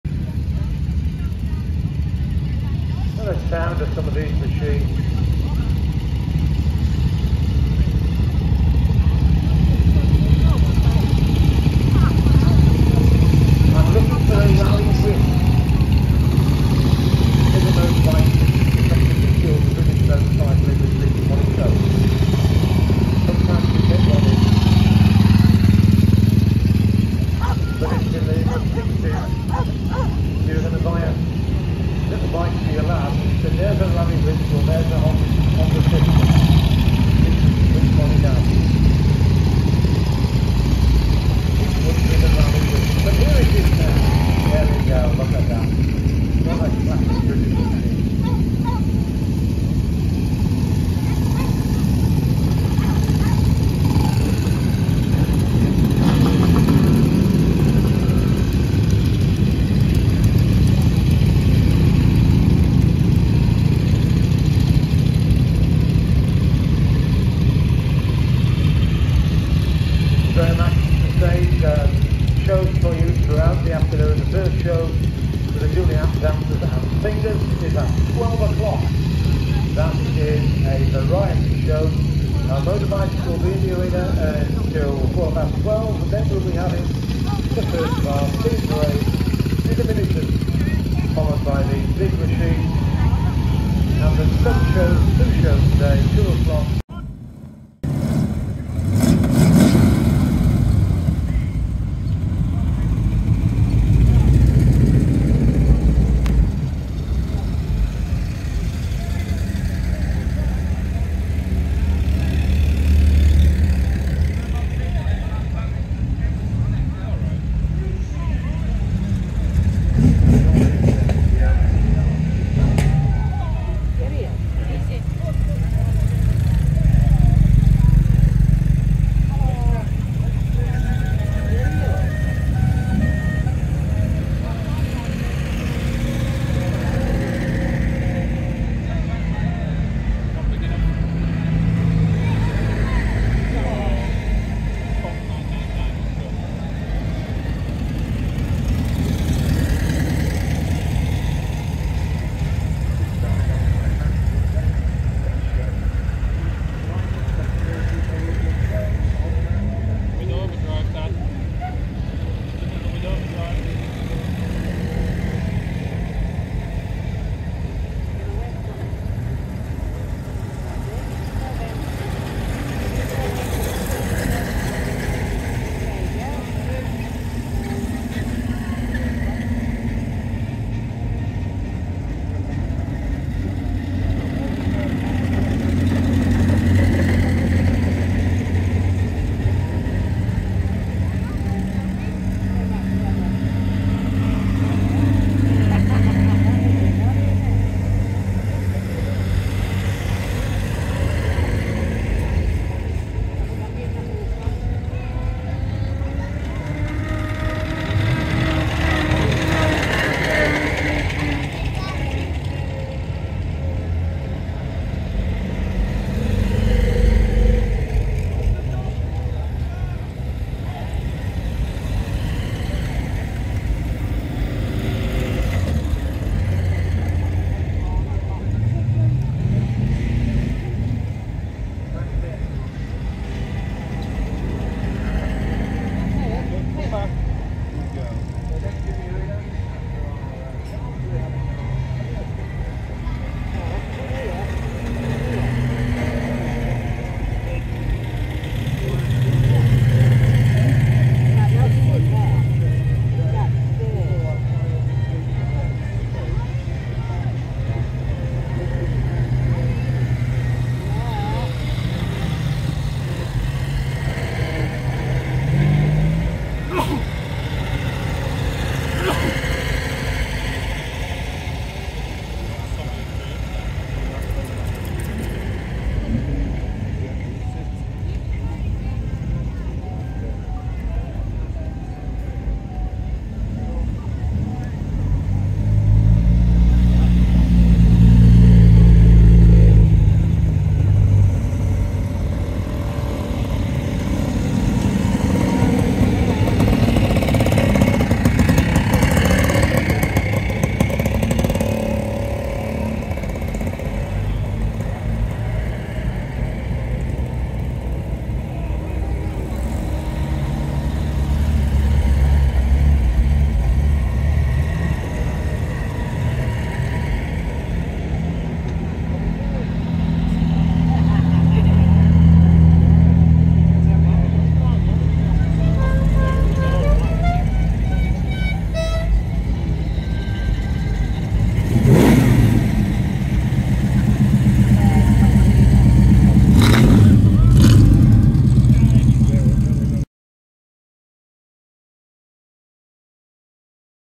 ***RAW FOOTAGE*** SCAMPSTON TRACTION ENGINE RALLY 2023 Vintage Cars & Motorbikes
Scampston Hall Traction Engine Rally 2023.
We attended this rally the first time after the last few years of not attending, we came up in my dad's unimog 1300l. We made it very late on the friday night after setting of late & then breaking down on the way too with a pipe that blew off the turbo but we made it!  It was such a hit with everyone and commented how nice and unusual it is! we will definitely be coming next year and also attending more outdoor events, they put such a great show on for everyone.

This short video is 1 of 6  from our weekend away at Scampston hall rally.

We now have TikTok!